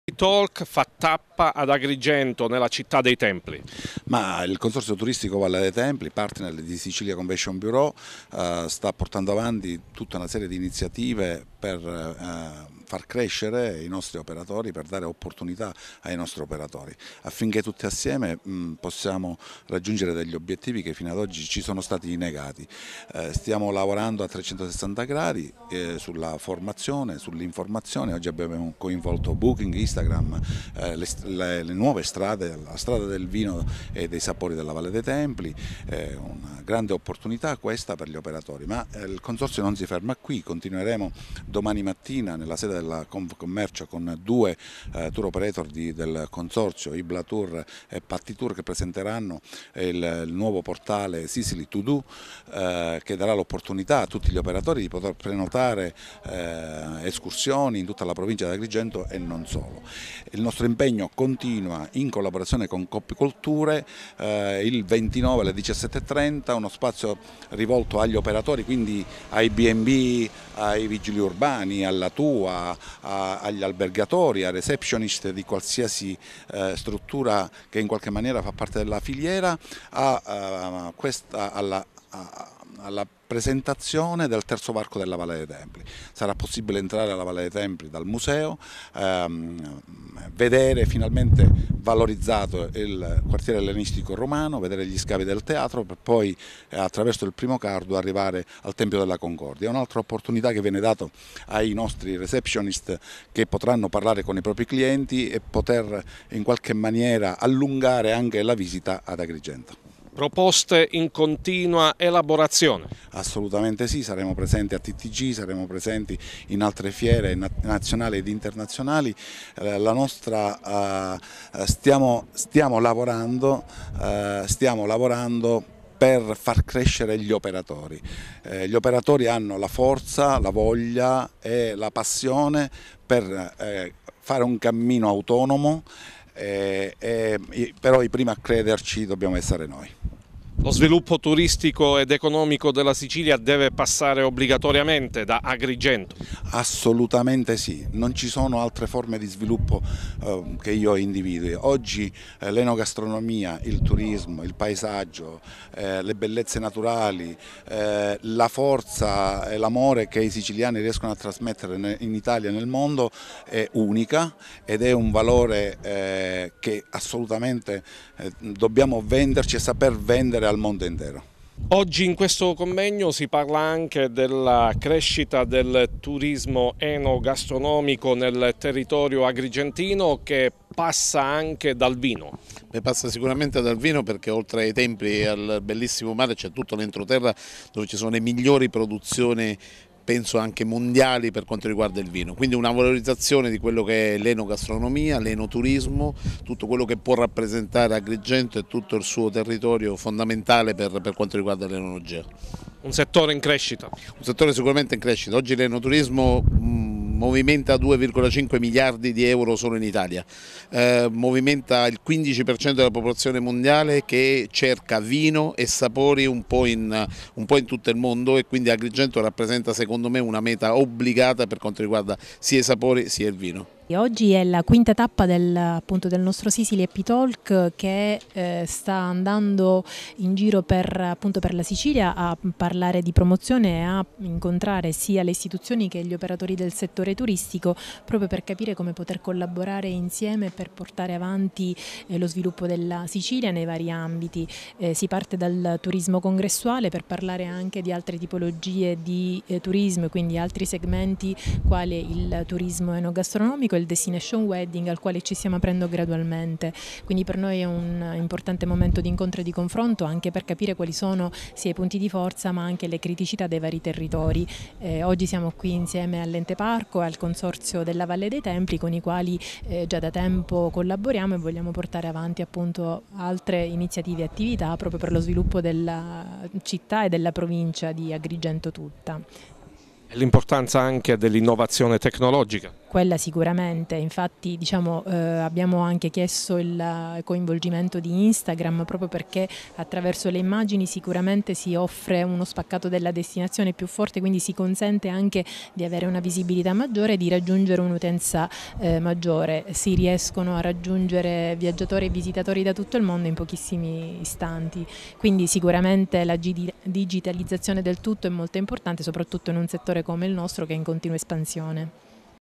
Agrigento: 3^edizione "Sicily Happy Talk"
Ad Agrigento su iniziativa di “Sicilia convention bureau” e del Consorzio turistico Valle dei Templi si è svolta la terza edizione di “Sicily Happy Talk” . Le interviste